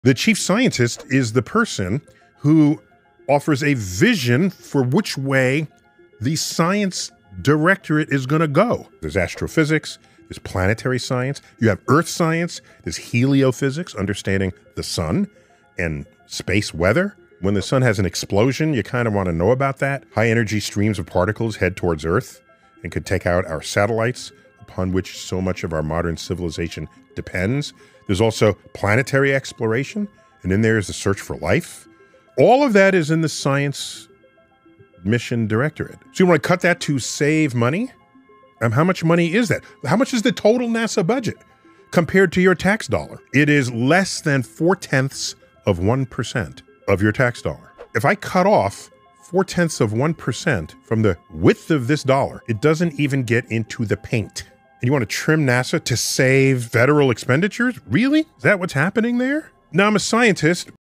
Why NASA’s Chief Scientist Matters | The Brain Behind Space Exploration
Have you ever wondered who shapes NASA’s biggest space missions and groundbreaking discoveries? In this video from Astro Minds, we reveal the crucial role of NASA’s Chief Scientist—the visionary guiding the agency’s scientific direction and interplanetary goals.

🧠 Inside This Video:

What does NASA’s Chief Scientist actually do?

How they influence missions to Mars, the Moon, and beyond

The link between science leadership and space innovation

Real examples of how science strategy shapes exploration

Why this role is critical to NASA’s success in the space race

📡 From coordinating planetary science to directing space research priorities, this is the untold story of NASA’s scientific mastermind.